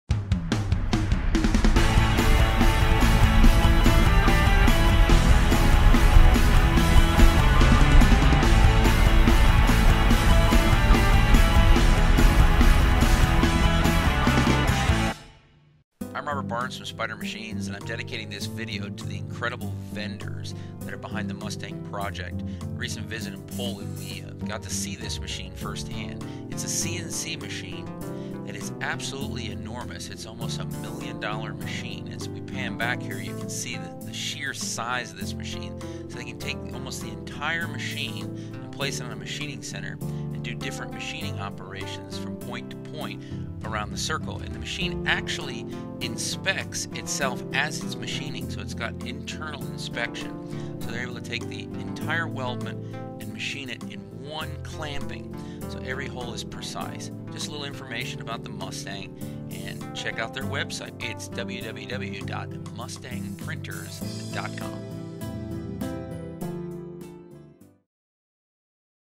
Spider Machines How-to - Mustang Machining - How it Works
Robert Barnes of Spider Machines shows a How to of the Mustang Machining operations on a Million Dollar CNC Machining Center in Poland